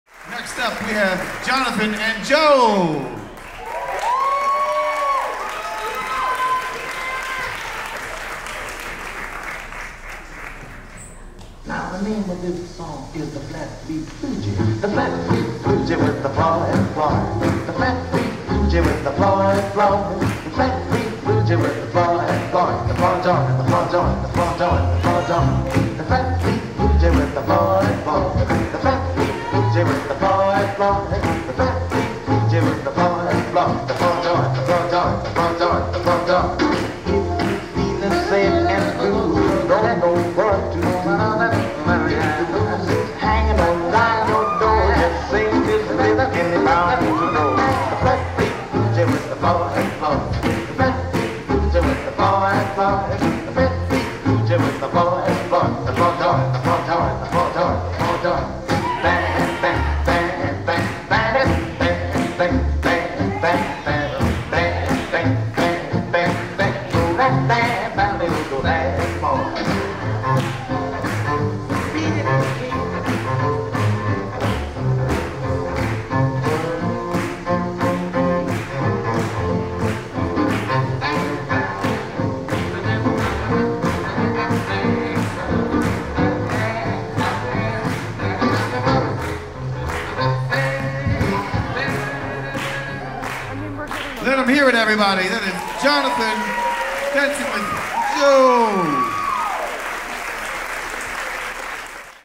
Jonathan & Jo - Pro-Am Strictly Finals - ILHC 2022
As first seen at ILHC 2022. The International Lindy Hop Championships takes place, in person, each year in New York City. for more information.

🎥ILHC 2022 Video Team:
Producer/Director: Brett Dahlenburg
Co-Producer/Camera Operator: Rachel Dahlenburg
Lead Camera Operator/Co-Producer: Ansley Sawyer
Camera Operators: Rafał Pustelny & Kevin Nguyen